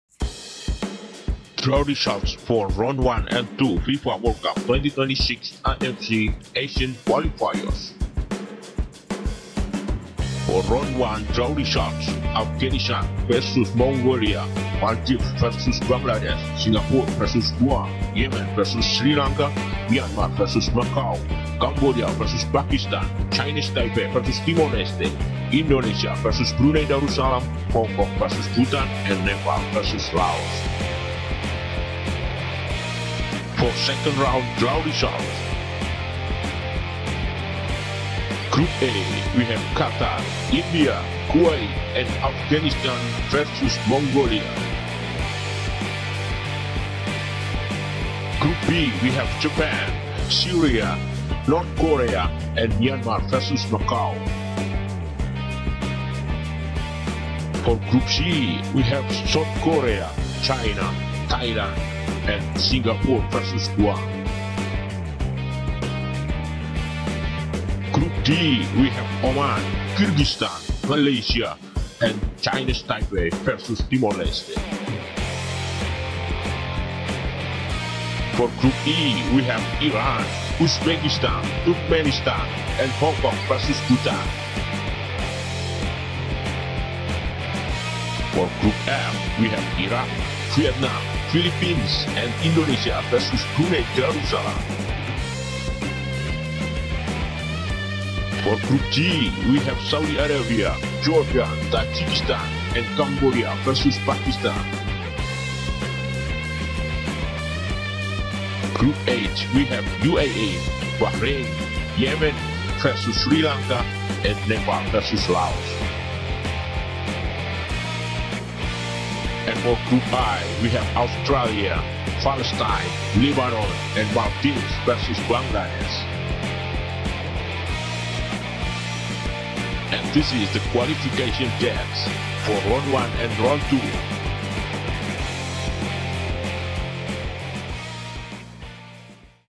FIFA World Cup 2026 AFC Qualifiers Round 2 Draw Results | AFC Asian Cup 2027 Qualification
Group A
Qatar, India, Kuwait, Afghanistan/Mongolia